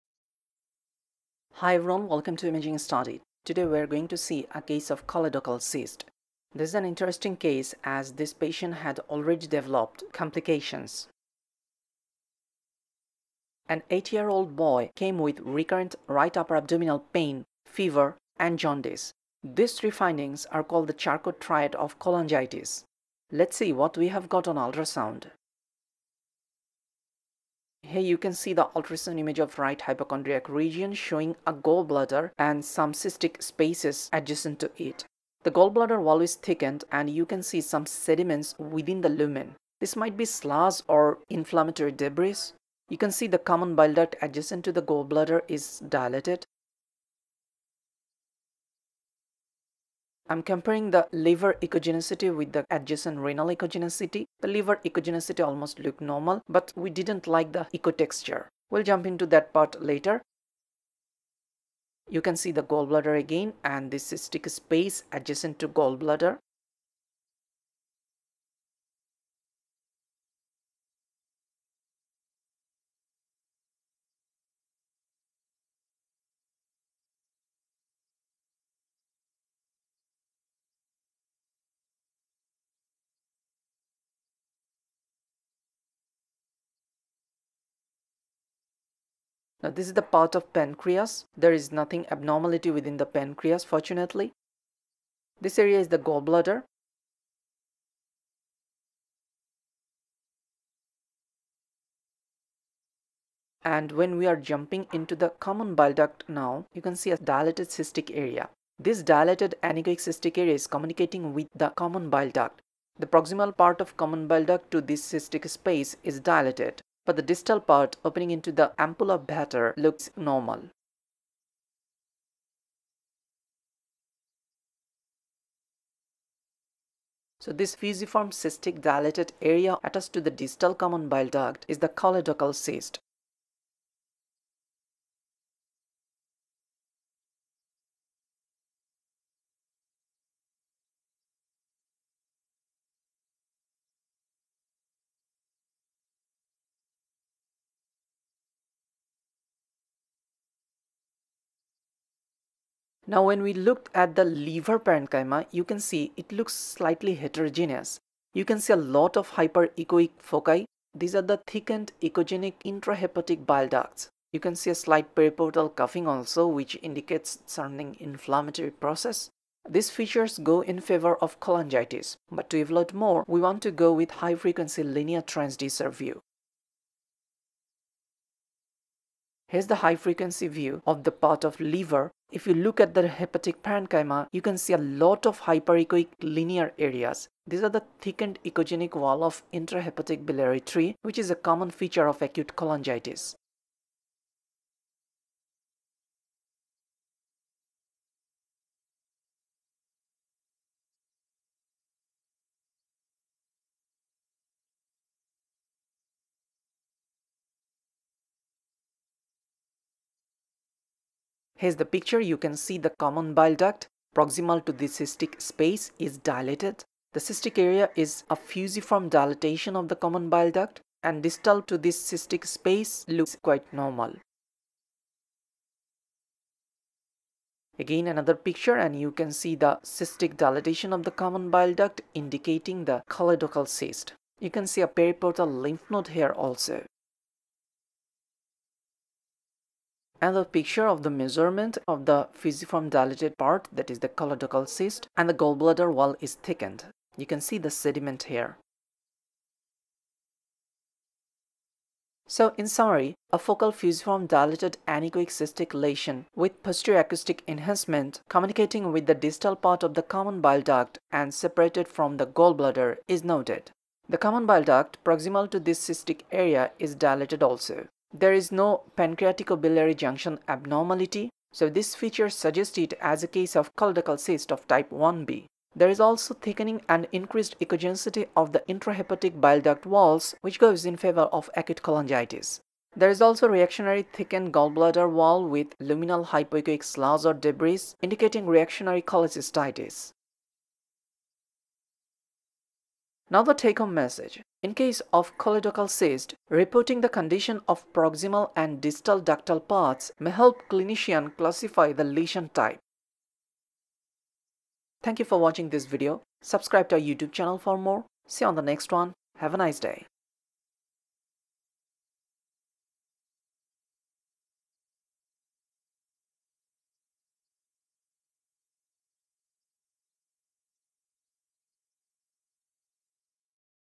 Choledochal Cyst with Cholangitis & cholecystitis || Ultrasound || Case 272
Clinical Features:
An 8-year-old boy came with
- Recurrent right upper abdominal pain.
- Fever
- Jaundice

Ultrasound Features:
- A focal fusiform dilated anechoic cystic lesion with posterior acoustic enhancement, communicating with the distal part of the common bile duct & separated from the gallbladder. The common bile duct, proximal to this cystic area, is dilated. No pancreaticobiliary junction (APBJ) abnormality. (Choledochal cyst - Type Ib)
- Thickening & increased echogenicity of the intrahepatic bile duct walls. (Acute Cholangitis)
- Thickened gallbladder wall with luminal hypoechoic sludge or debris. (Reactionary Cholecystitis)

Remember:
In case of choledochal cyst, reporting the condition of proximal & distal ductal parts may help classify the lesion type.